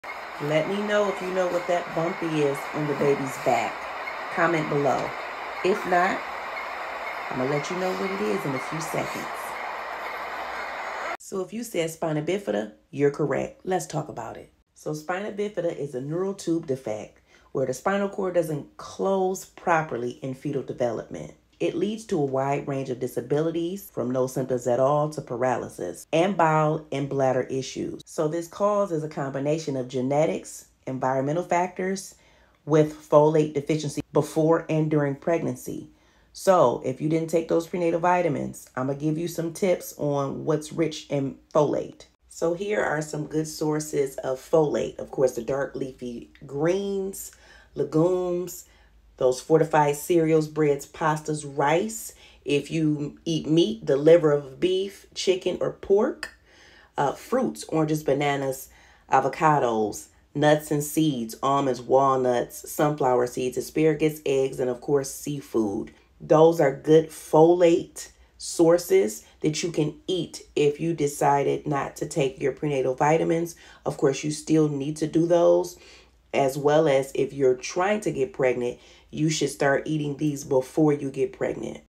Newborn shows
In today’s video, we’ll discuss Spina Bifida, a congenital condition that affects the spinal cord and brain development in newborns. We’ll cover what causes it, how it’s diagnosed, the types of spina bifida, and the essential role nurses play in caring for these infants and supporting their families through treatment and recovery.

👶 Whether you’re a nurse, nursing student, or parent wanting to learn more, this video breaks down the condition in a compassionate and educational way.